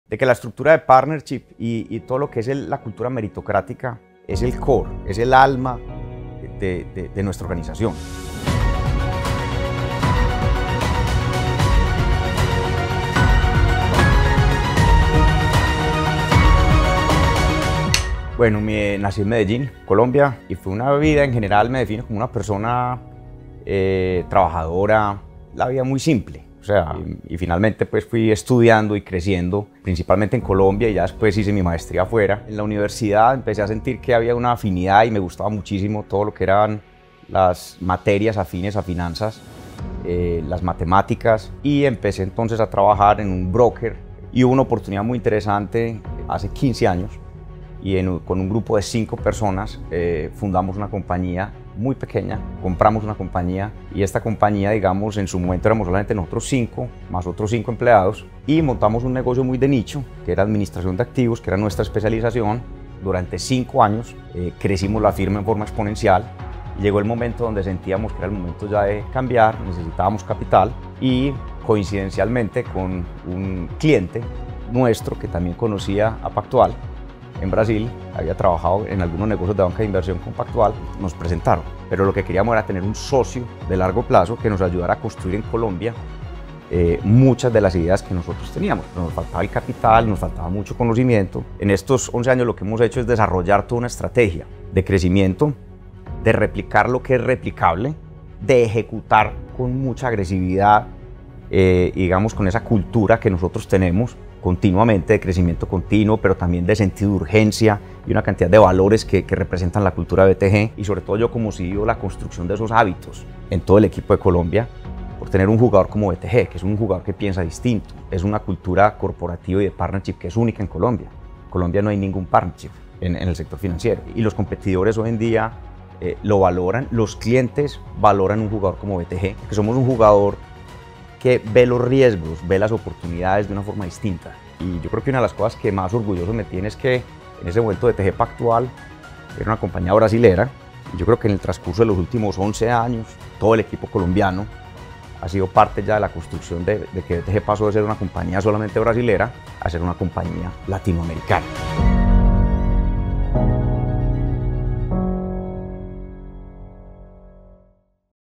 Conheça a história de Juan Rafael Pérez | BTG Pactual 40 anos
Neste novo episódio da série BTG Pactual 40 anos, conheça a história de Juan Rafael Pérez, CEO do BTG Pactual Colômbia. No vídeo, Pérez descreve a sua trajetória no mercado financeiro, até o seu encontro com o BTG Pactual. Ele destaca a importância da cultura de partnership meritocrática para o Banco e para os clientes. Assista ao vídeo para saber mais!

En este nuevo episodio de la serie BTG Pactual 40 años, conoce la historia de Juan Rafael Pérez, CEO de BTG Pactual Colombia. En el video, Pérez narra su trayectoria en el mercado financiero hasta llegar a BTG Pactual. Destaca la importancia de la cultura meritocrática de partnership tanto para el banco como para los clientes. ¡Mira el video para obtener más información!